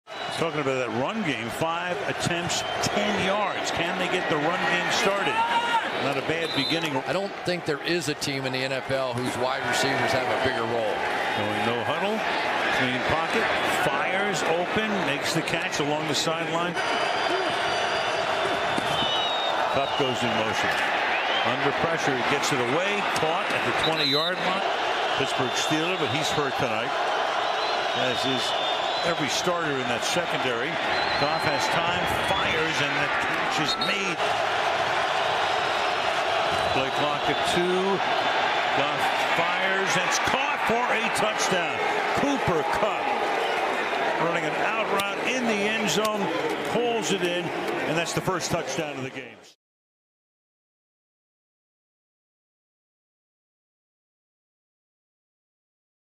Rams Open 2nd Half w/ TD Drive!
Jared Goff connects with Cooper Kupp for the first touchdown of the night! The Los Angeles Rams take on the Cleveland Browns during Week 3 of the 2019 NFL season.